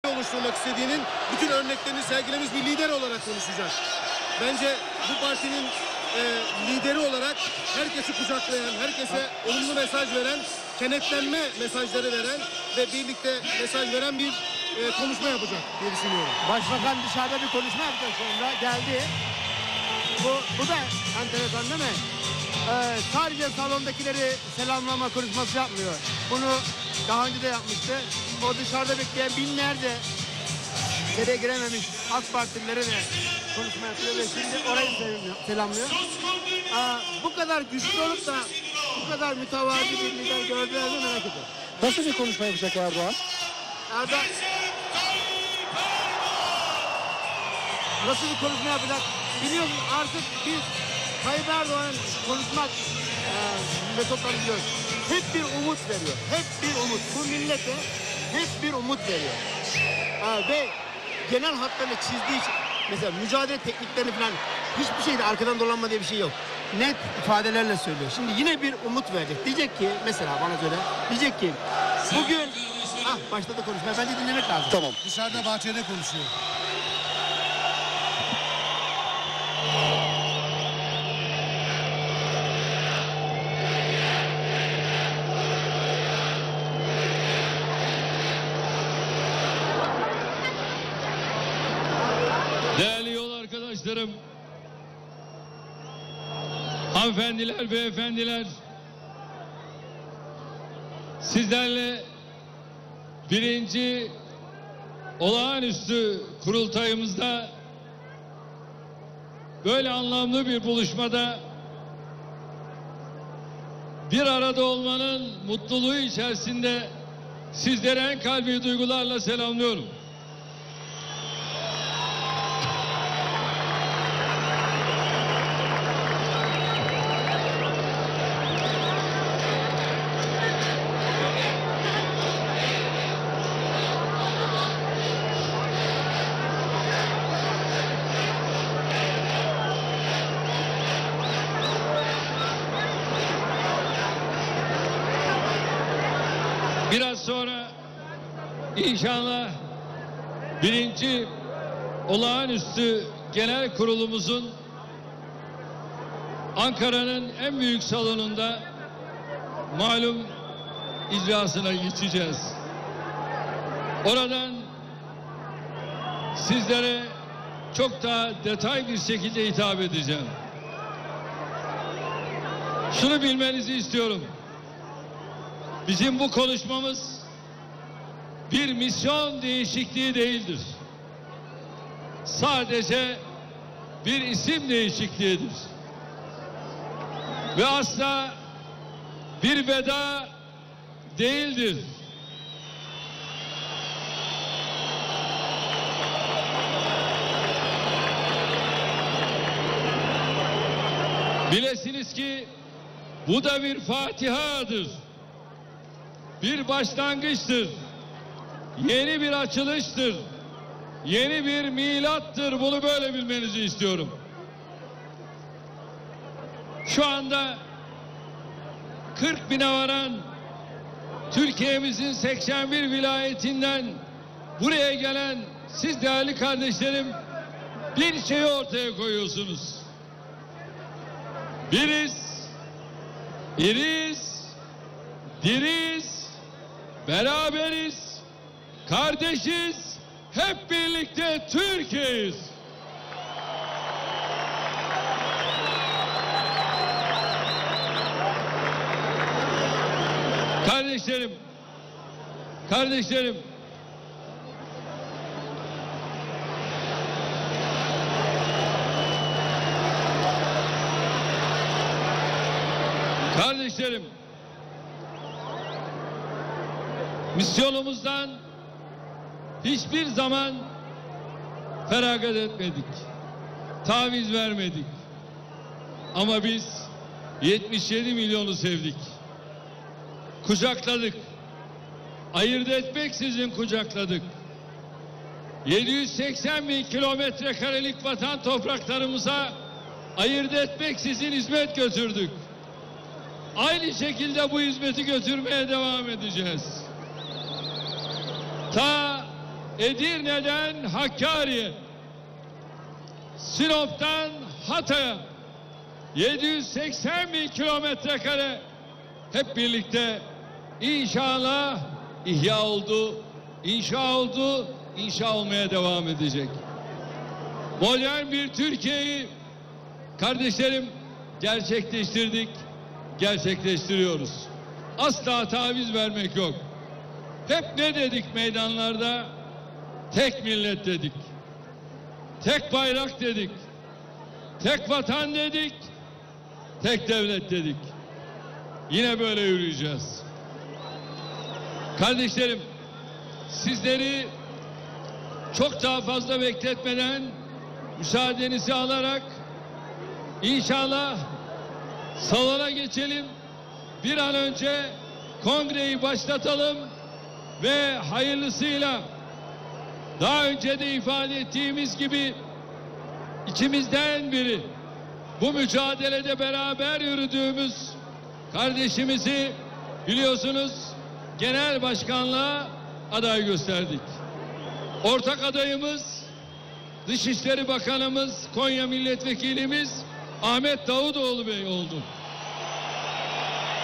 ERDOĞAN'DAN DUYGUSAL VEDA...
AK PARTİ 1. olağan kongresi öncesi Genel Başkanlığı Ahmet Davutoğlu'na devreden Recep Tayyip Erdoğan, salon dışında bekleyen kalabalığa seslendi.